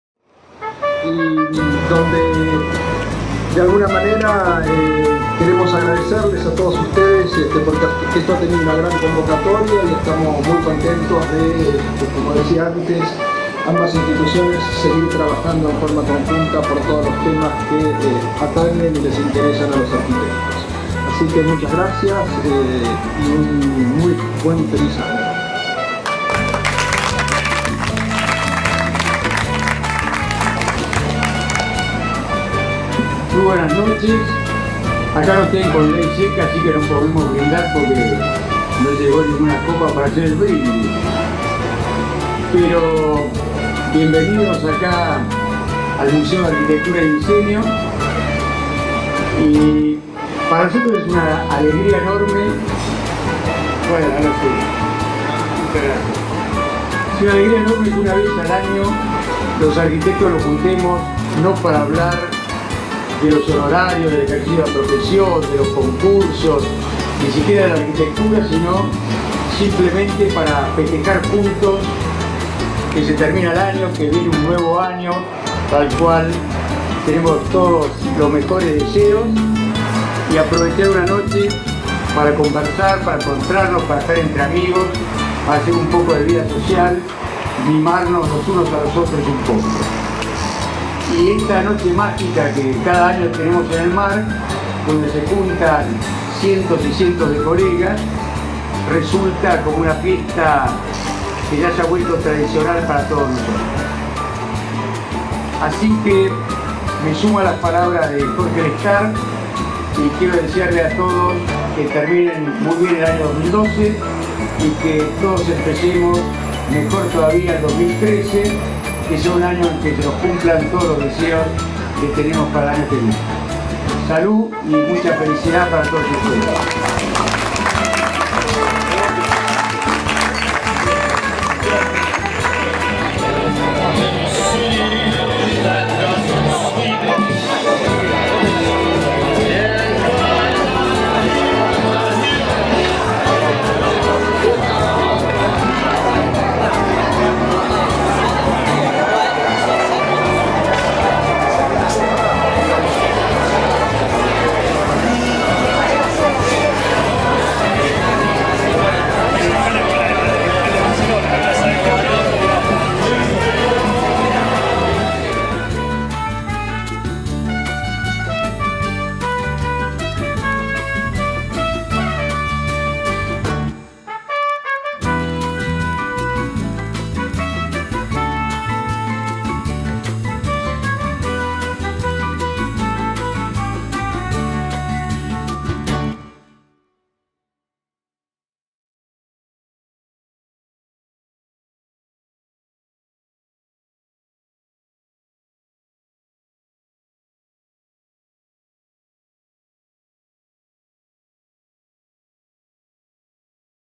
Fin de Año 2012 CPAU y SCA
El pasado martes 11 de diciembre los arquitectos y amigos nos reunimos, en el Marq. Museo de Arquitectura y Diseño Julio Keselman, para tomar unas copas y brindar por un nuevo año, organizado por el SCA Sociedad Central de Arquitectos y el CPAU Consejo Profesional de Arquitectura y Urbanismo, Buenos Aires Argentina